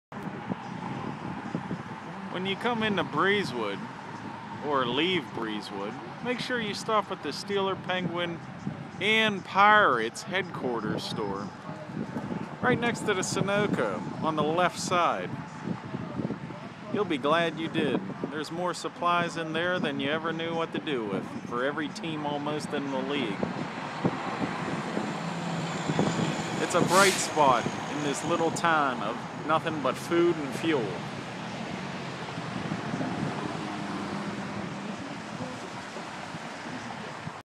Breezewood Pa.
Steelers  Penguins Pirates Store in Breezewood Pa. Formally Crawfords Museum.Across the street from the Gateway Plaza.